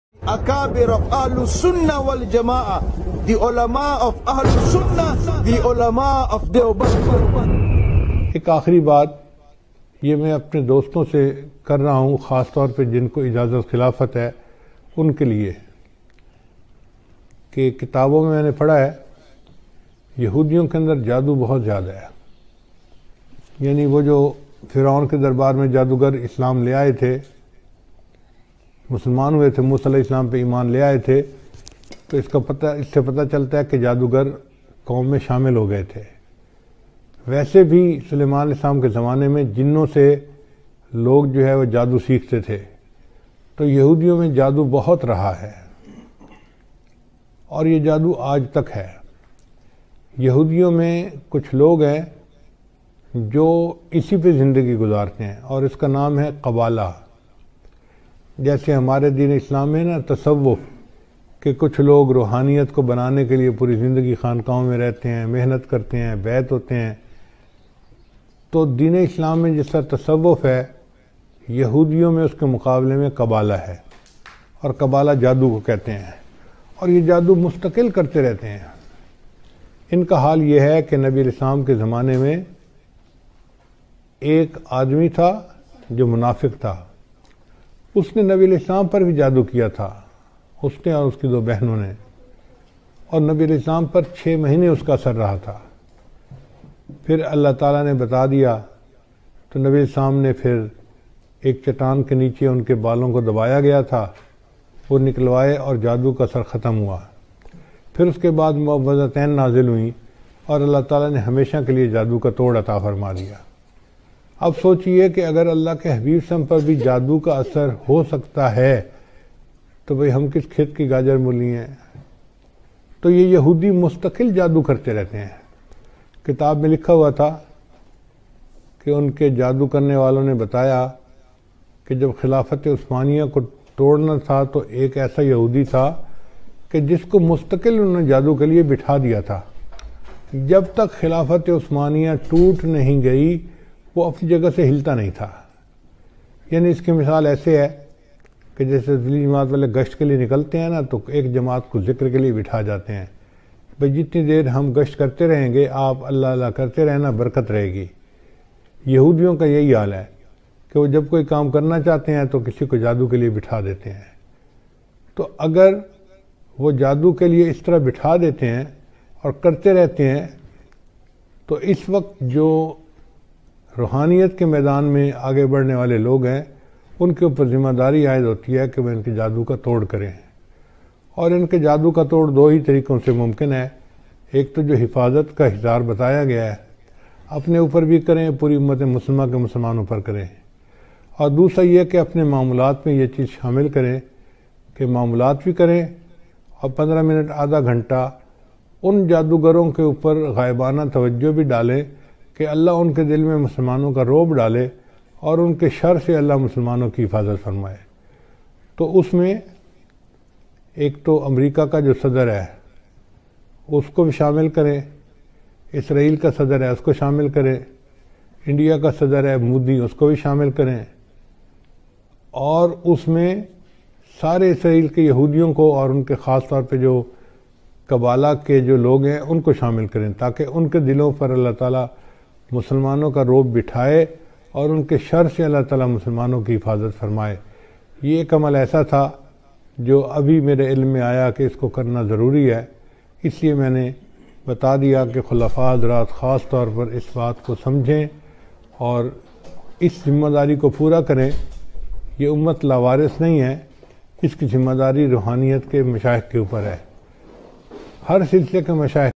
Hazrat ji Maulana Hafiz Peer Zulfiqar Ahmed Naqusbandi DB | Ke Ek Aham Appil
محبوب العلماء والصلحا ء حضرت جی   شیخ ذوالفقار  احمد نقشبندی دامت برکاتہم کی ایک اہم اپیل اپنے خلفاء حضرات سے جو شاید حضرت نے پہلی بار کہی حضرت نے نام زد متعین کیا ہے خلفاء حضرات. اپنے معمولات کے وقت غائبانہ  توجہ ڈالیں مسلمانوں کی حفاظت کیلۓ فتین کے شر سے بچانے کیلۓ  امریکہ کا صدر اسرائیل کا صدر وہاں کےیہودی اور تیسرا ہے مودی